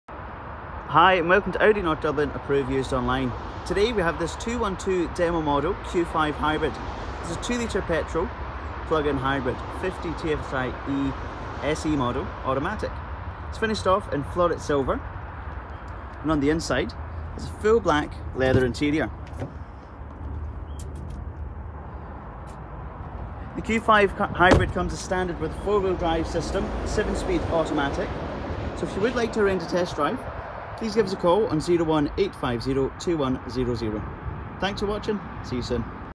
212D6445 - 2021 Audi Q5 50TFSIe QUATTRO SE S-T RRP 62,243 - SAVE 3,243 59,0...
Here is a video of our 212D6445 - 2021 Audi Q5 50TFSIe QUATTRO SE S-T RRP 62,243 - SAVE 3,243 59,000. Please contact sales to arrange a test drive today. Audi NorthPhone: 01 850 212D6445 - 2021 Audi Q5 50TFSIe QUATTRO SE S-T RRP 62,243 - SAVE 3,243 59,000100